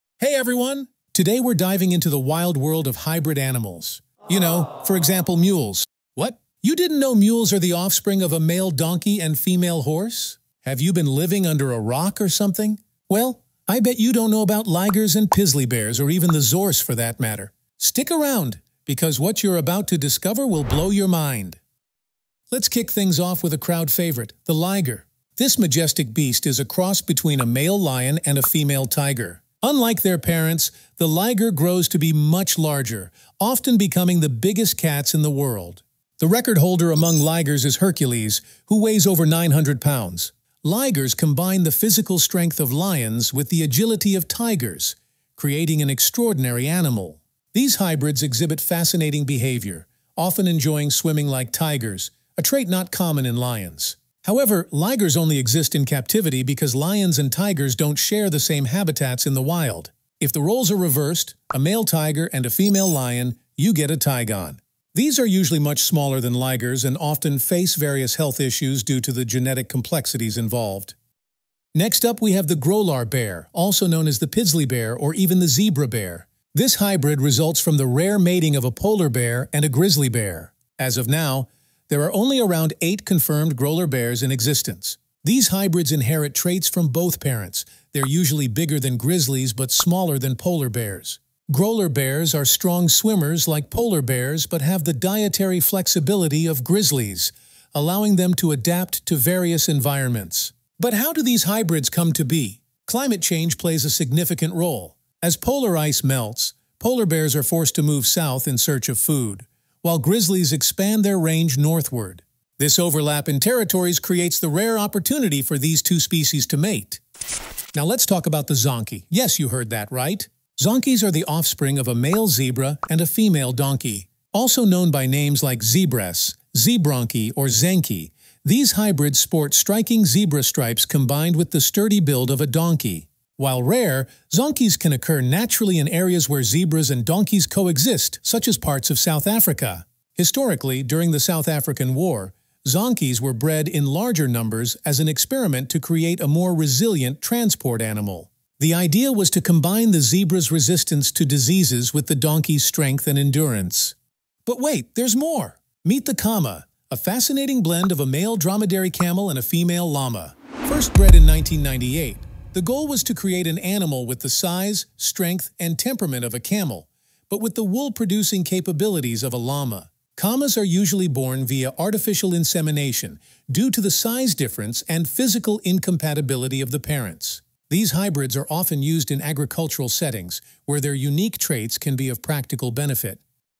Hybrid Animals that Actually EXIST
Discover the fascinating world of real hybrid animals! From the powerful liger to the adorable wholphin, this video explores nature's most intriguing crossbreeds. We'll dive into the science behind these unique creatures, their characteristics, and the controversy surrounding their existence. Perfect for animal lovers and science enthusiasts alike!